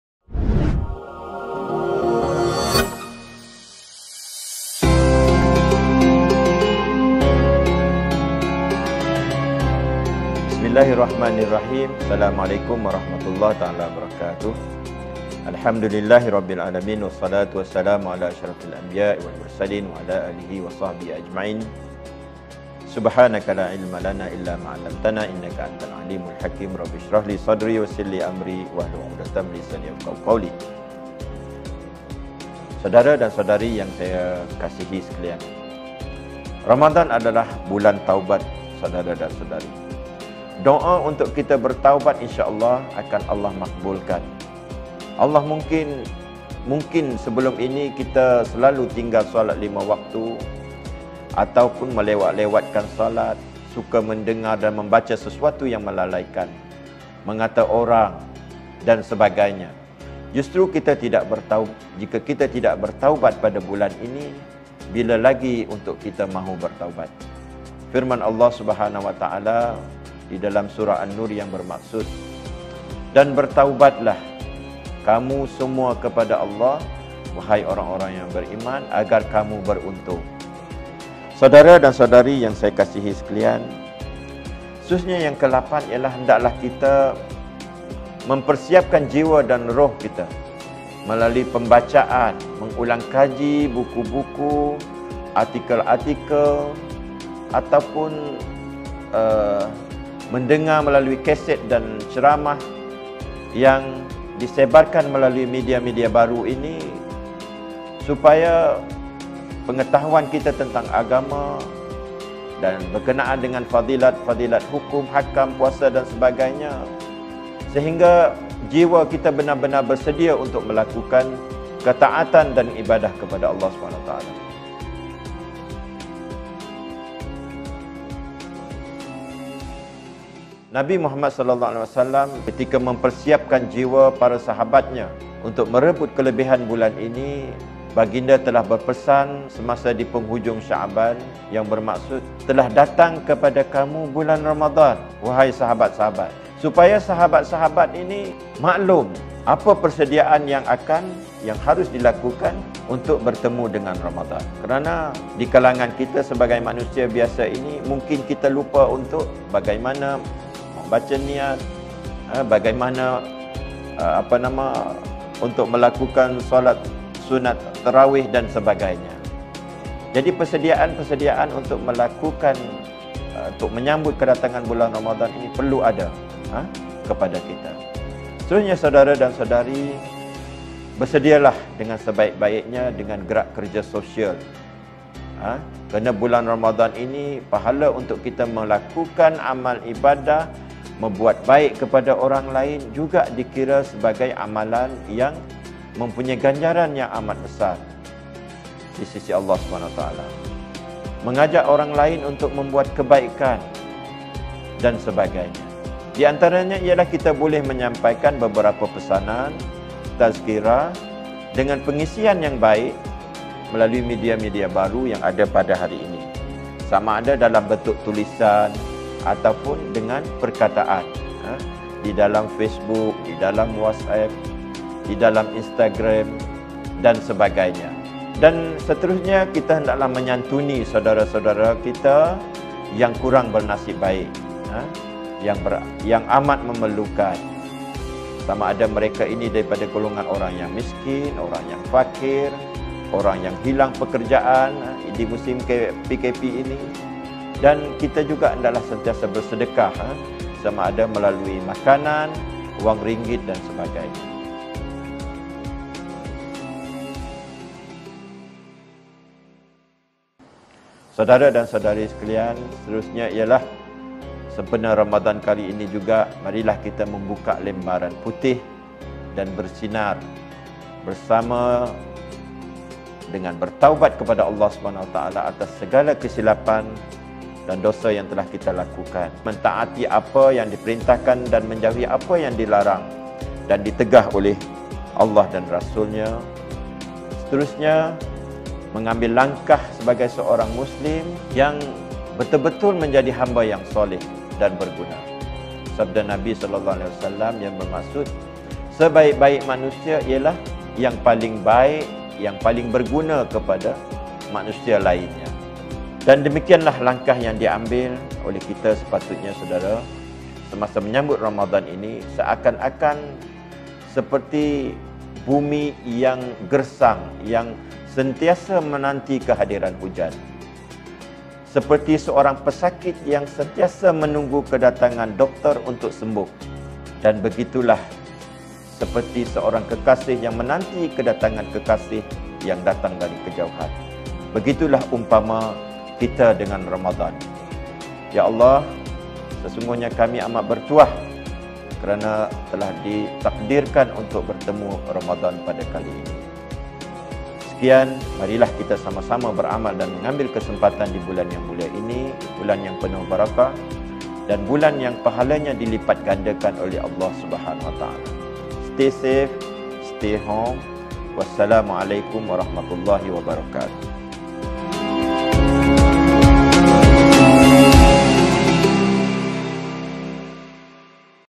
SIRI 6 TAZKIRAH RAMADAN KAMEK
Tazkirah Ramadan Kamek @JapenSarawak Siri 6
Program terkini Japen Sarawak, Tazkirah Ramadan Kamek @JapenSarawak bersama Ustaz Sukarno bin Saini, Ketua Penolong Pengarah Bahagian Dakwah, Jabatan Agama Islam Sarawak sempena bulan Ramadan Al-Mubarak bagi mengimarahkan suasana sambutan Ramadan dalam Perintah Kawalan Pergerakan (PKP) Covid 19. Tajuk tazkirah yang di sampaikan "Mengejar Keberkatan dan Keampunan di bulan Ramadan" Siri 6.
Sama-sama kita saksikan video ini.